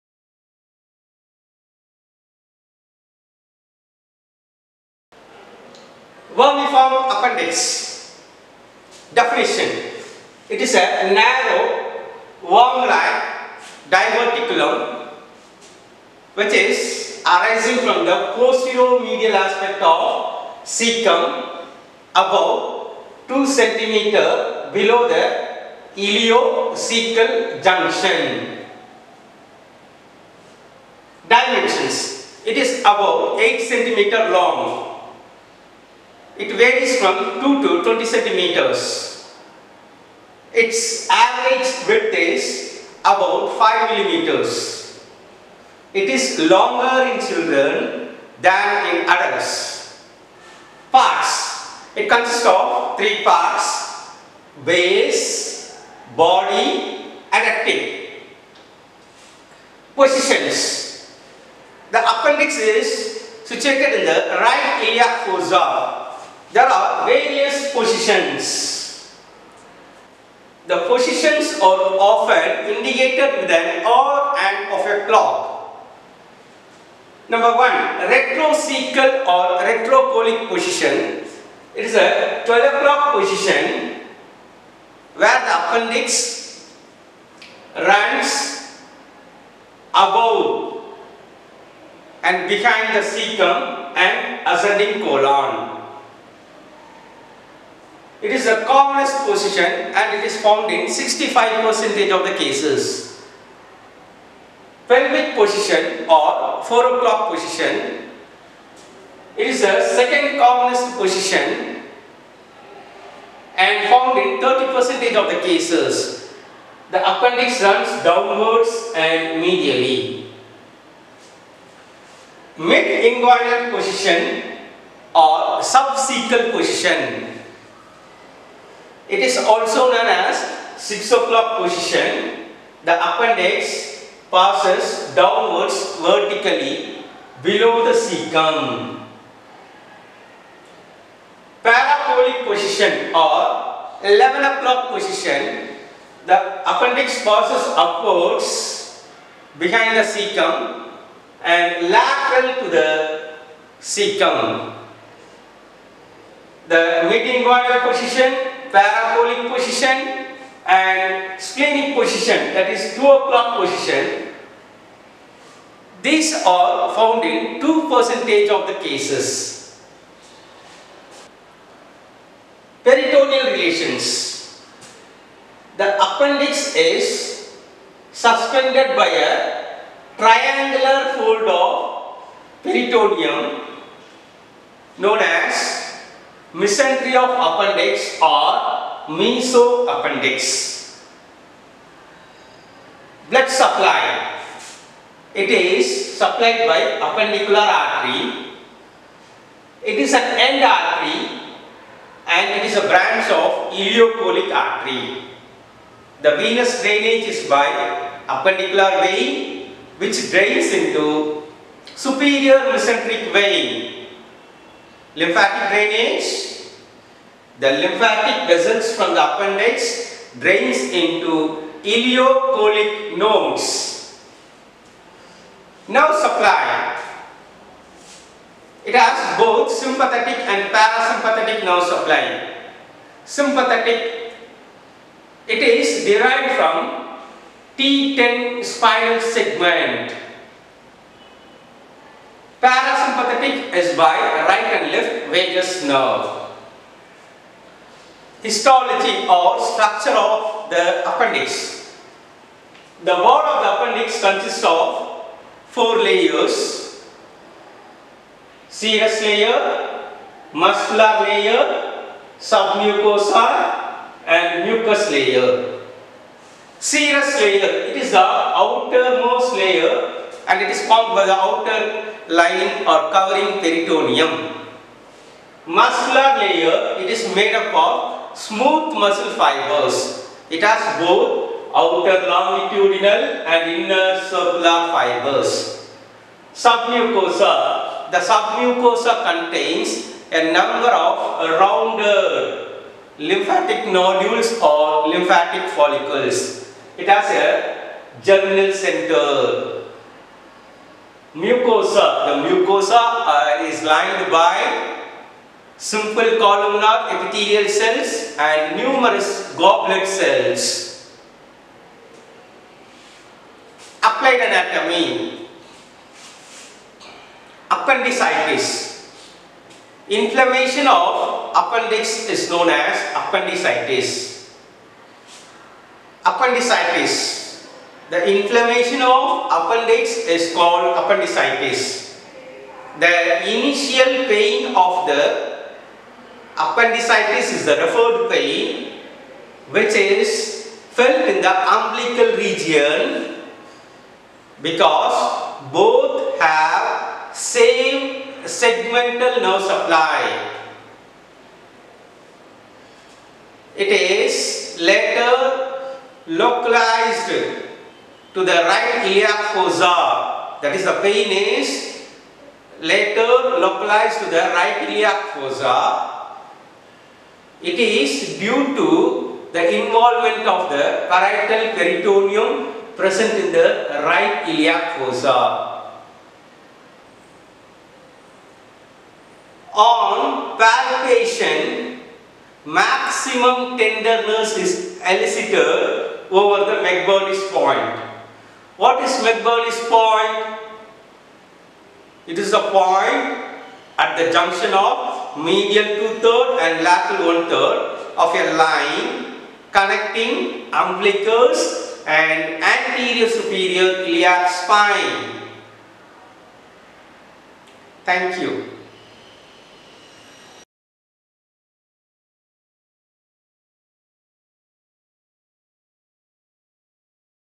Appendix / Vermiform Appendix
Appendix / Vermiform Appendix: Definition, Parts, Positions, Blood & Nerve Supply, Histology and Applied Anatomy